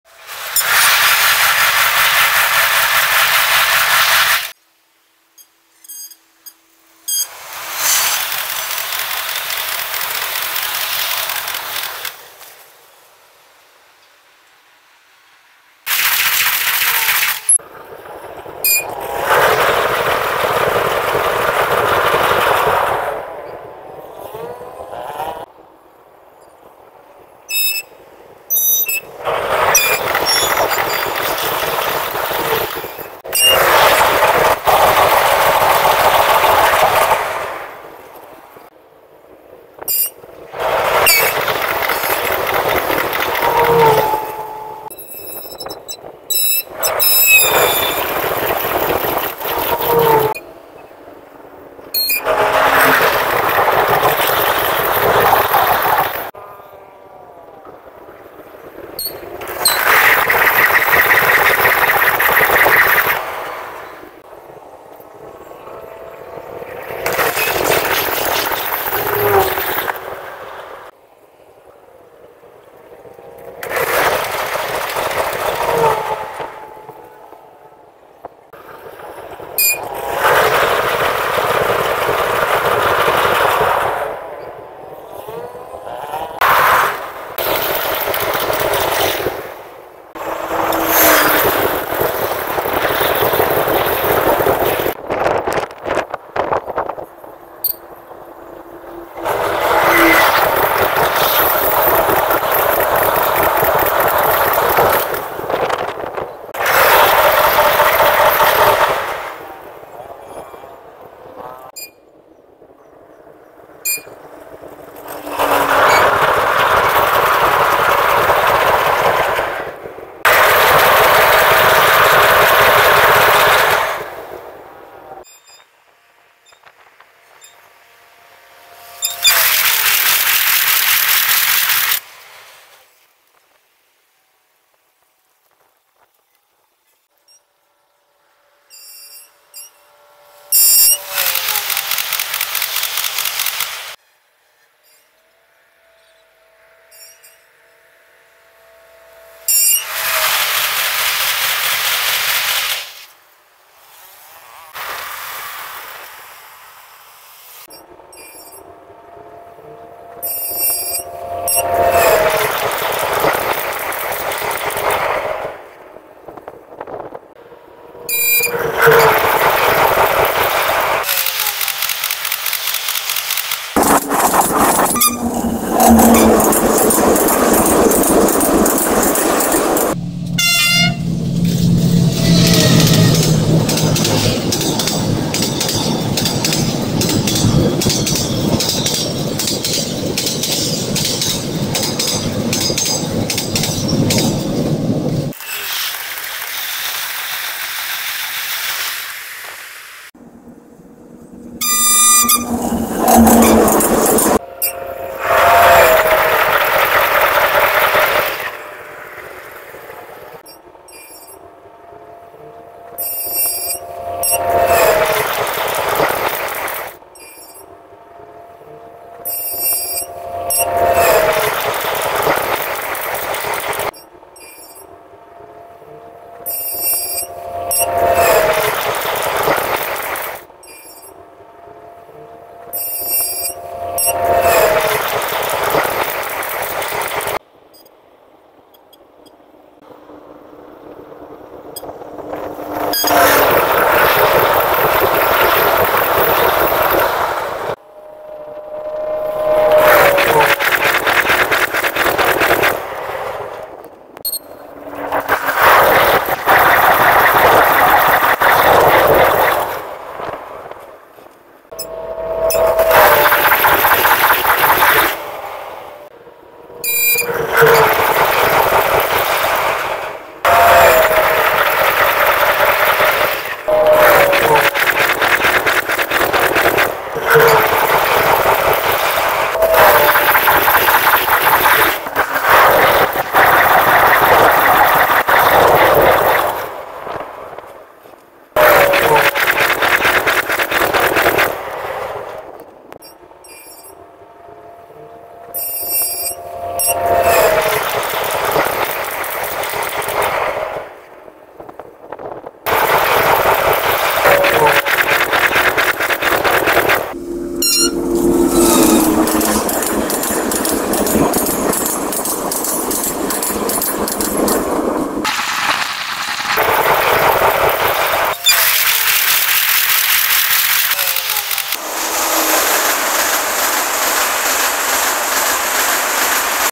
High speed Vietnamese trains
dodinhthanh video / Vietnamese trains run fastly through Phong Le 1 bridge , Da nang city . Filmed & published by Thanh Do Dinh ( Vietnamese : Đỗ Đình Thành ) . Two locomotive types : D19Es ( blue ) & D20Es ( grey & had logo )